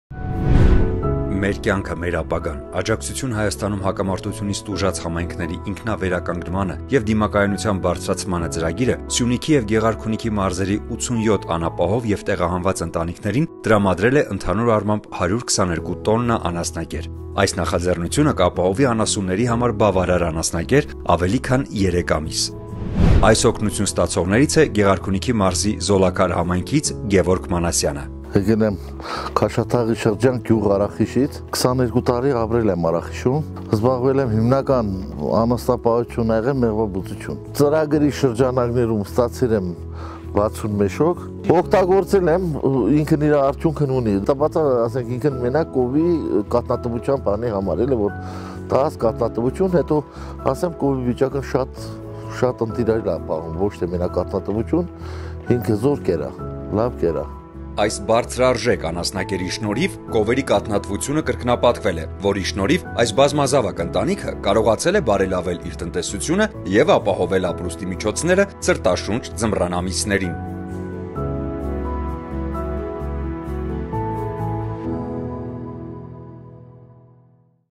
Gevorg Manasyan
After the escalation of hostilities in and around Nagorno-Karabakh in late 2020, many families were forced to move to remote and mountainous border regions of Armenia. Households that brought their cattle with them as a vital source of food and income for their families encountered serious difficulties along the way. The situation intensified with the arrival of the harsh winter and raising prices of hay and fodder.

The “Our lives, our future. Supporting self-recovery and resilience of conflict-affected communities in Armenia” project provided 87 vulnerable and displaced families in Syunik and Gegharkunik regions with 122 tons of fodder. This initiative will provide sufficient fodder for their animals for more than 3 months.
Gevorg Manasyan from the Zolakar community of Gegharkunik region is one of the recipients of this assistance.

Watch this short video clip about Gevorg.

“Our lives, our future. Supporting self-recovery and resilience of conflict-affected communities in Armenia” project is funded by the European Union Humanitarian Aid and implemented by the UNDP. EU Civil Protection & Humanitarian Aid European Union in Armenia